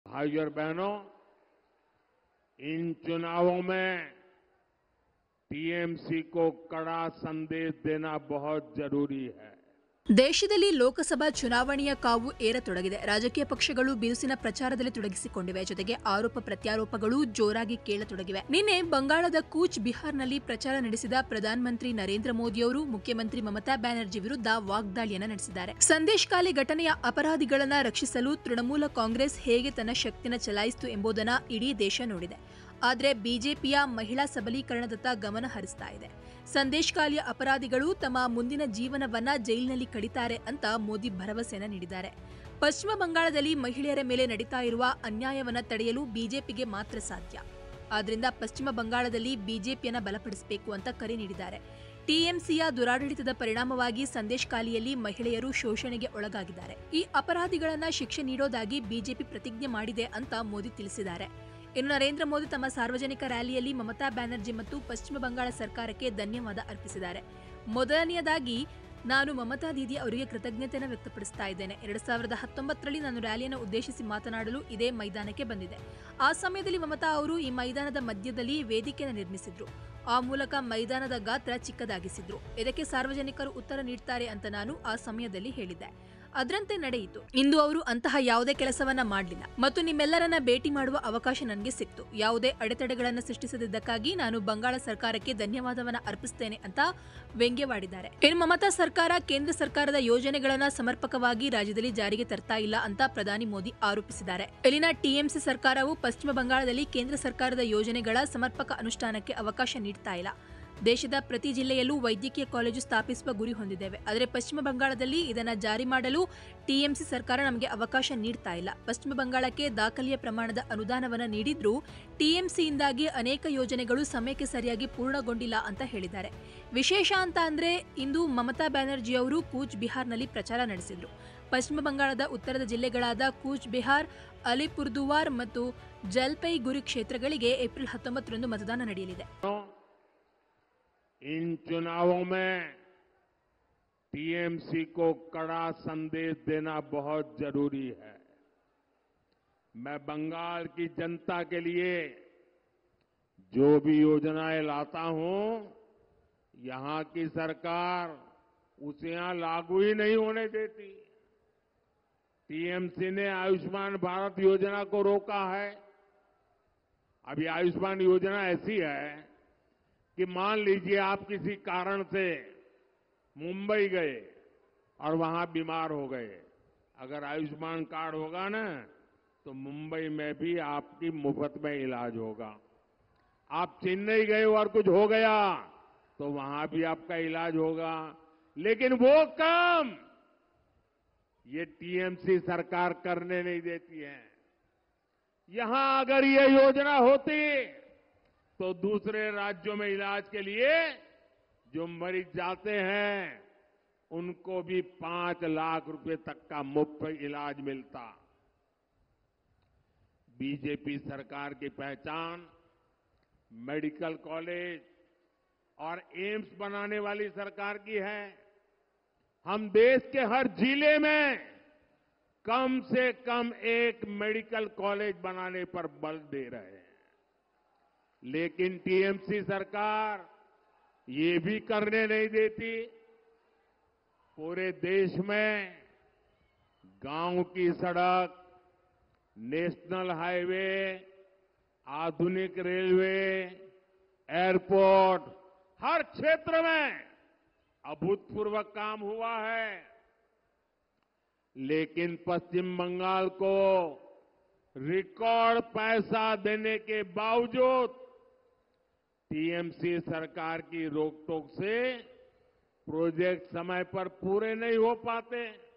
ಸಂದೇಶಖಾಲಿಯ ಅಪರಾಧಿಗಳಿಗೆ ಕಠಿಣ ಶಿಕ್ಷೆ ಒದಗಿಸುವುದೇ ಬಿಜೆಪಿಯ ಗುರಿ ಎಂದ PM Modi
ದೇಶದಲ್ಲಿ ಲೋಕಸಭಾ ಚುನಾವಣೆ (Lok Sabha Election 2024)ಯ ಕಾವು ಏರತೊಡಗಿದೆ. ರಾಜಕೀಯ ಪಕ್ಷಗಳು ಬಿರುಸಿನ ಪ್ರಚಾರದಲ್ಲಿ ತೊಡಗಿಸಿಕೊಂಡಿವೆ. ಜತೆಗೆ ಆರೋಪ-ಪ್ರತ್ಯಾರೋಪಗಳೂ ಜೋರಾಗಿ ಕೇಳಿ ಬರತೊಡಗಿವೆ. ಗುರುವಾರ (ಏಪ್ರಿಲ್‌ 4) ಬಂಗಾಳದ ಕೂಚ್ ಬೆಹಾರ್ (Cooch Behar)ನಲ್ಲಿ ಪ್ರಚಾರ ನಡೆಸಿದ ಪ್ರಧಾನ ಮಂತ್ರಿ ನರೇಂದ್ರ ಮೋದಿ (Narendra Modi) ಅವರು ಮುಖ್ಯಮಂತ್ರಿ ಮಮತಾ ಬ್ಯಾನರ್ಜಿ (Mamata Banerjee) ವಿರುದ್ಧ ವಾಗ್ದಾಳಿ ನಡೆಸಿದ್ದಾರೆ.

The anticipation of the Lok Sabha Election 2024 is starting to rise in the country. Political parties are engaged in vigorous campaigning. In addition, accusations and counter-accusations are also heard loudly. On Thursday (April 4), Prime Minister Narendra Modi, who campaigned in Bengal's Cooch Behar, attacked Chief Minister Mamata Banerjee.

~HT.290~PR.160~ED.33~